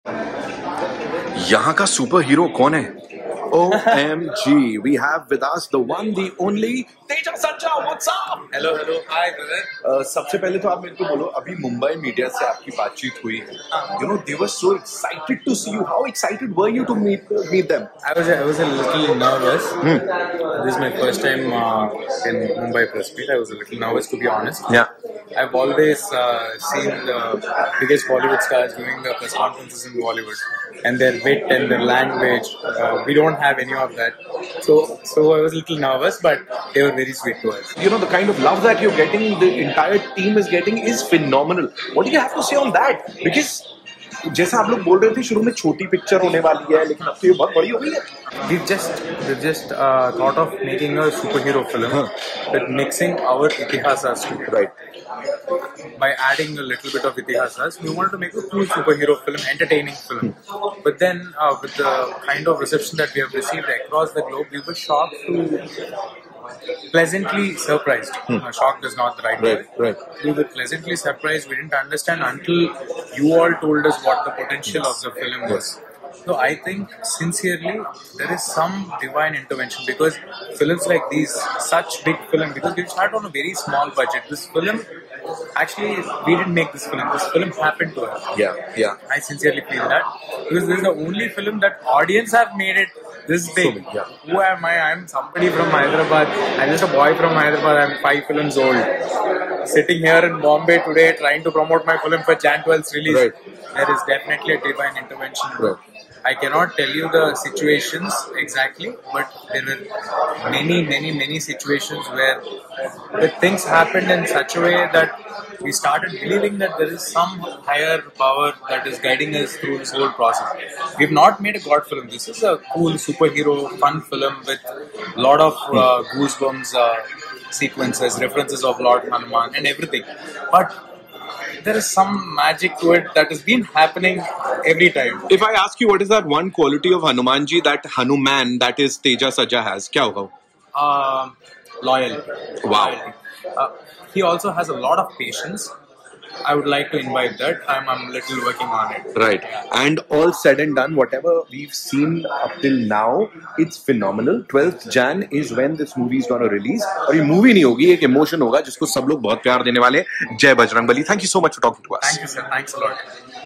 Teja Sajja Exclusive Interview- Hanuman Movie Actor - Hanuman Movie Hindi Dubbed, Chiranjeevi
here is a specials interaction with this star who's been around for quite some tie and we've all loved his performances, be it as a child artist or now as a superhero in this soon to release movie HANUMAN.
In this exclusive interview Teja Sajja talks about how cine lovers have made this movie so huge even before the release and what was it like playing this superhero that he does so amazingly.